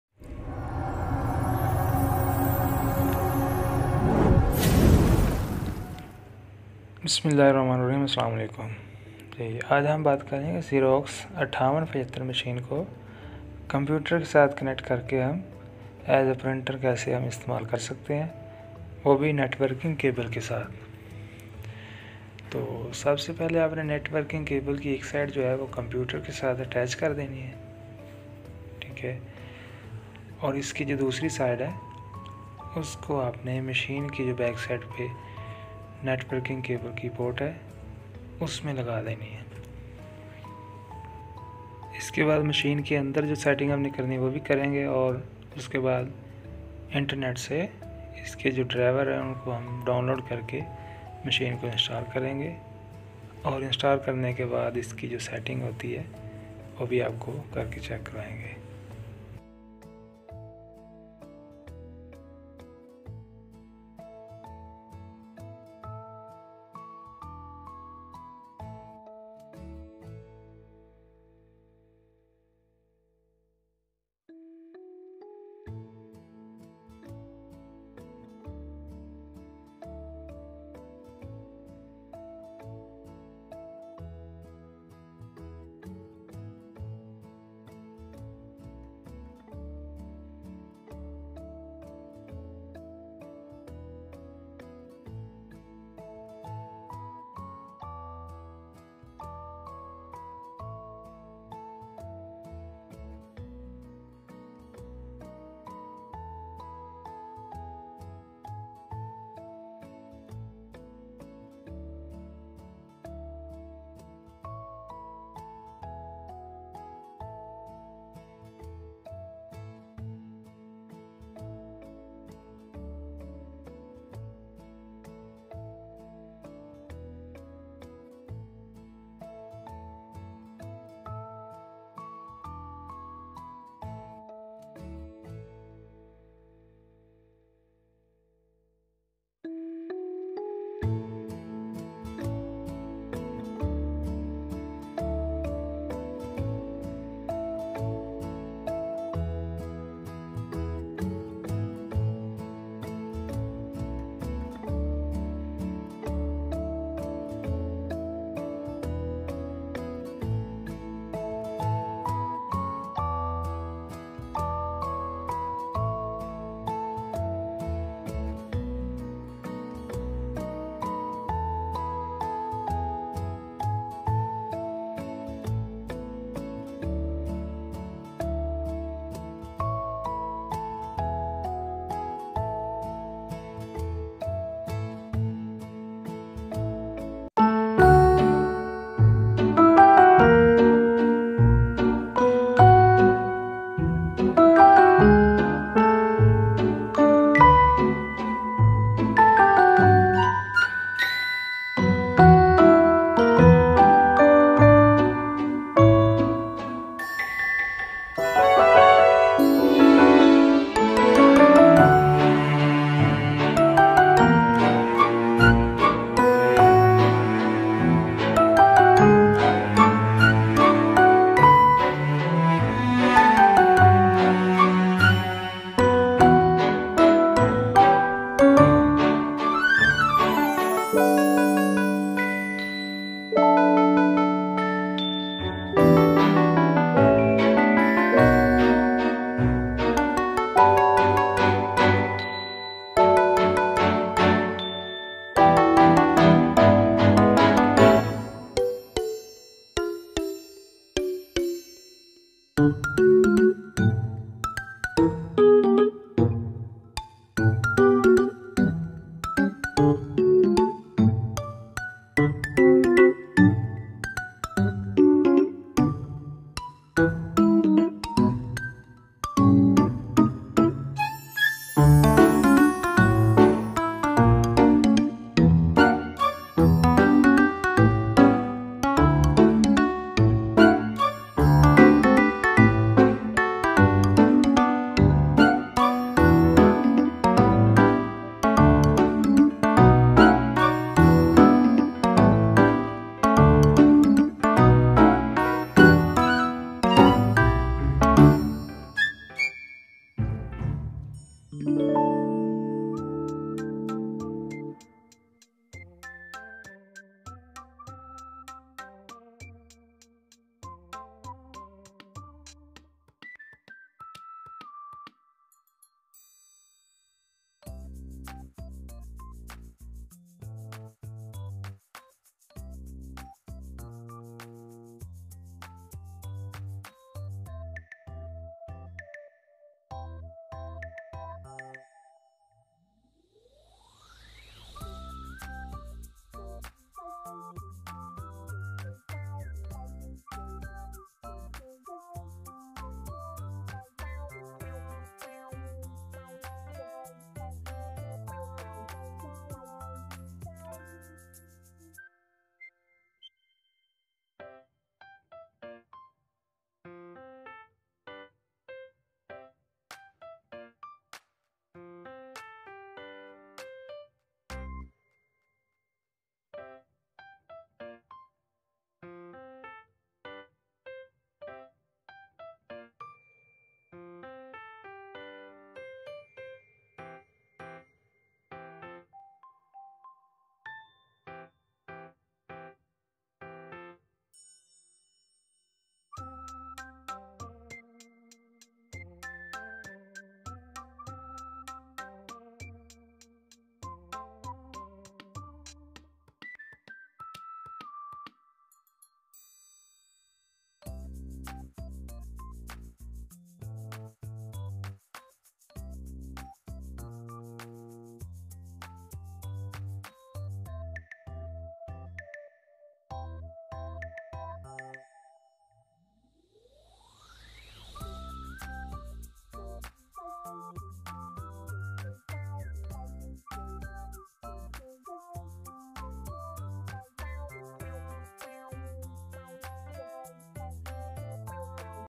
How to install Xerox 5875 with networking CABLE| Xerox ko computer sy kasy Conact kry
زیروکس 5875 انسٹال کرنے کا طریقہ
نیٹ ورکنگ کیبل کے ساتھ زیروکس 5875 کو کیسے انسٹال کریں
How to install Xerox 5875| Xerox masheen  ko kasy instal kry | Xerox ko computer sy kasy Conact kry